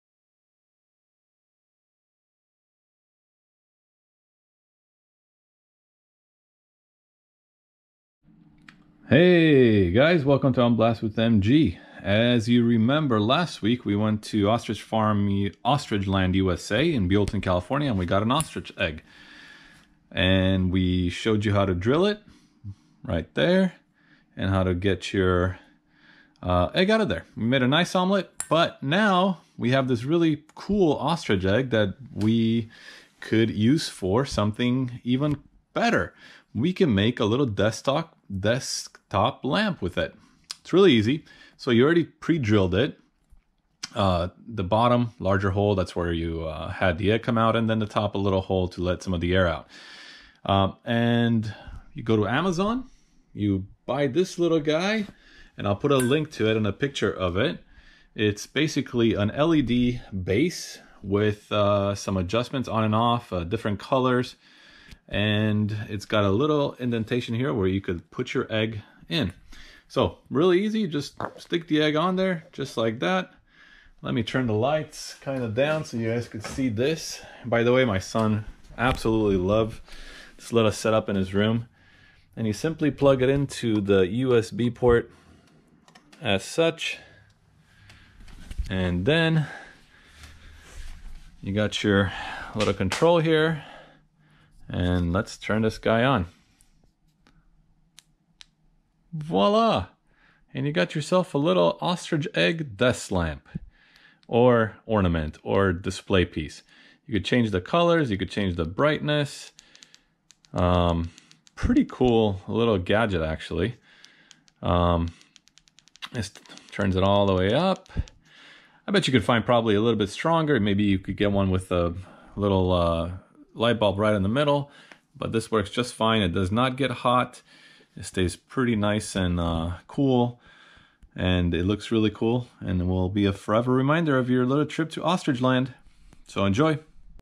DIY Ostrich Egg Lamp Display Base
This is the follow up video on making our Ostrich egg into a desk top lamp/display piece.  Our son absolutely loves the fact that not only did he get to eat the egg but now has a "DINOSAUR"  egg in his room.  Either way, the base is 11$ on amazon.

Eanatah 3 Colors Adjustable Display Base 12 LED Light Stand Base for 3D Crystals Glass Resin Art

Premium Ostrich Eggshell (Grade A, Large Size)